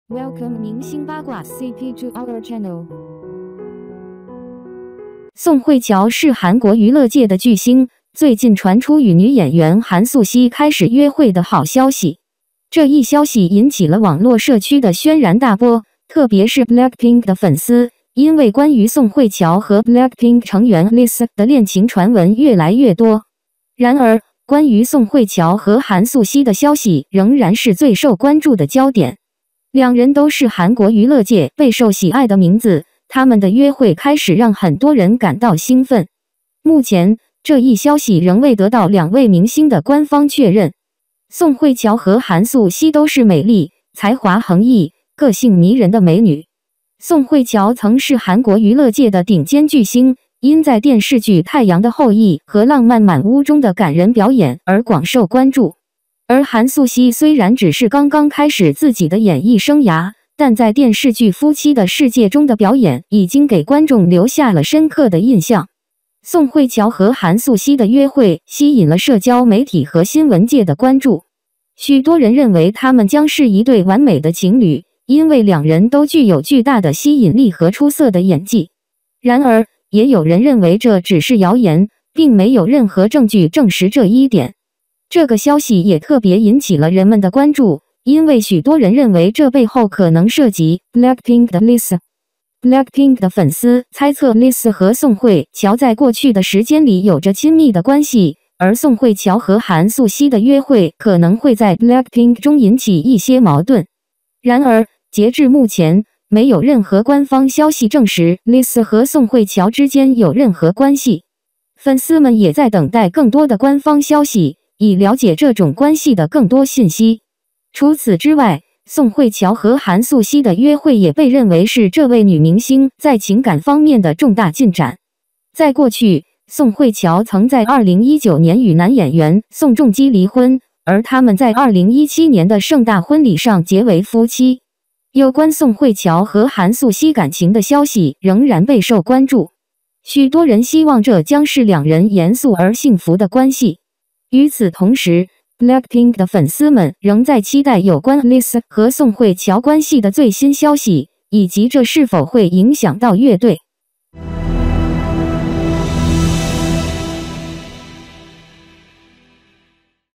宋慧乔因与韩苏熙的萌芽爱情而收到好消息？
宋慧乔是韩国娱乐界的巨星，最近传出与女演员韩素希开始约会的好消息。这一消息引起了网络社区的轩然大波，特别是BLACKPINK的粉丝，因为关于宋慧乔和BLACKPINK成员Lisa的恋情传闻越来越多。
然而，关于宋慧乔和韩素希的消息仍然是最受关注的焦点。两人都是韩国娱乐界备受喜爱的名字，他们的约会开始让很多人感到兴奋。目前，这一消息仍未得到两位明星的官方确认。
宋慧乔和韩素希都是美丽、才华横溢、个性迷人的美女。宋慧乔曾是韩国娱乐界的顶尖巨星，因在电视剧《太阳的后裔》和《浪漫满屋》中的感人表演而广受关注。而韩素希虽然只是刚刚开始自己的演艺生涯，但在电视剧《夫妻的世界》中的表演已经给观众留下了深刻的印象。